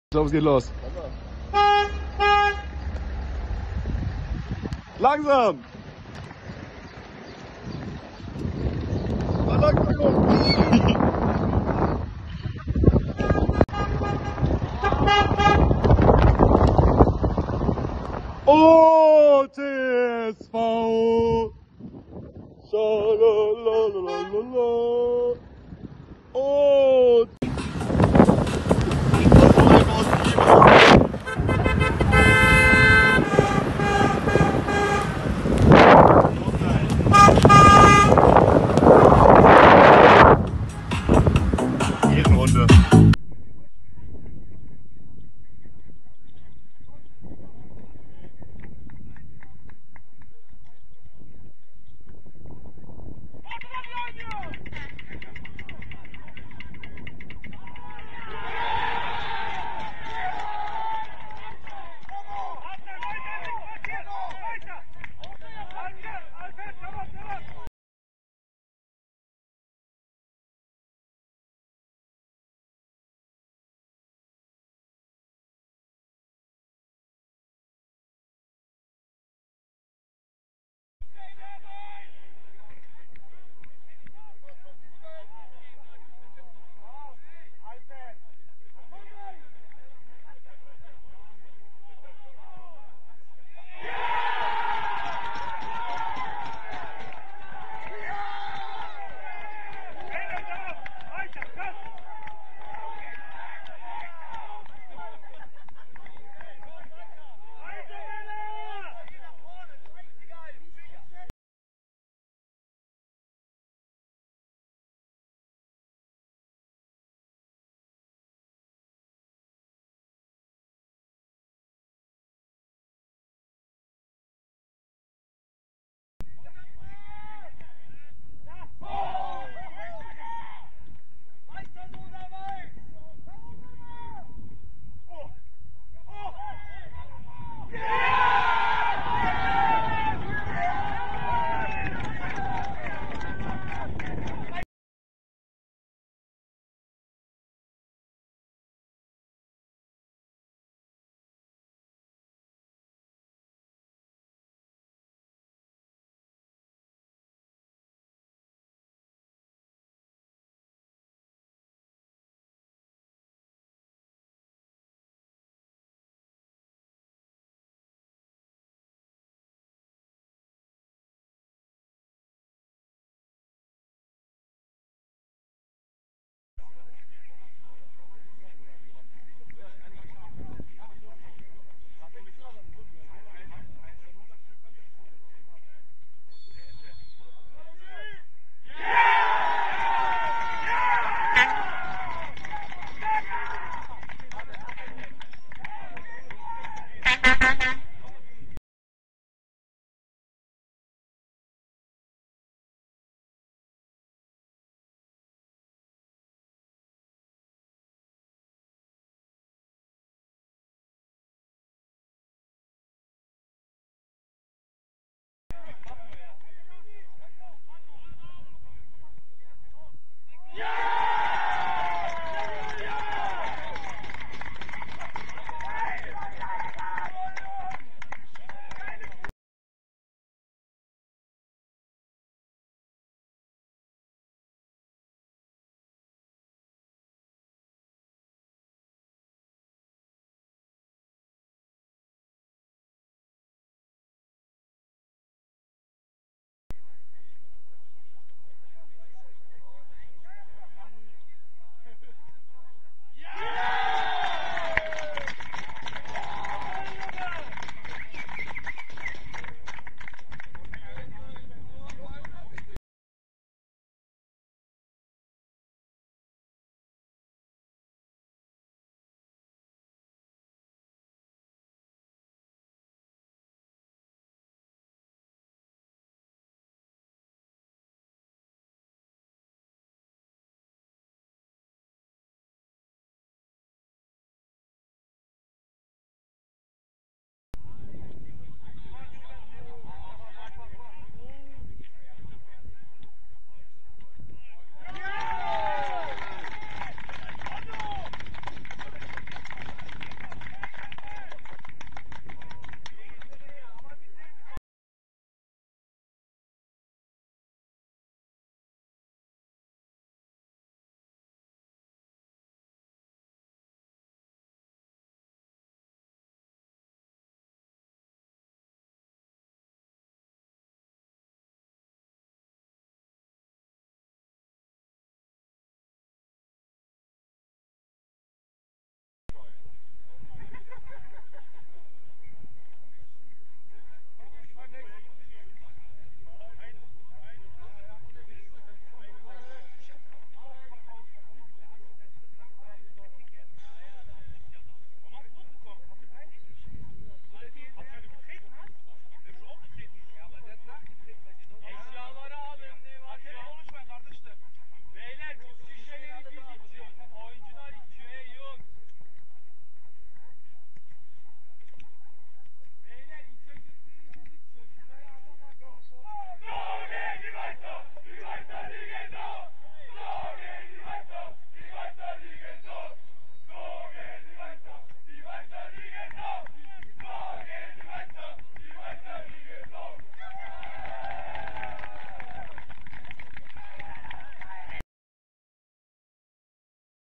TSV Weiss II vs. SC Meschenich II 15.05.2022
Die Tore zu unserem Spiel gegen den bis dato amtierenden Tabellenführer.
Mit einer überragenden Mannschaftsleistung konnten hier am Sonntag die Gäste aus Meschenich mit 6:0 zuhause geschlagen werden.

Der Sieg bedeutet gleichermaßen das man wieder auf Platz1 der Kresiliga D Rhein Erft ist und so dem gesetzten Ziel - Aufstieg wieder ein Stück näher gekommen ist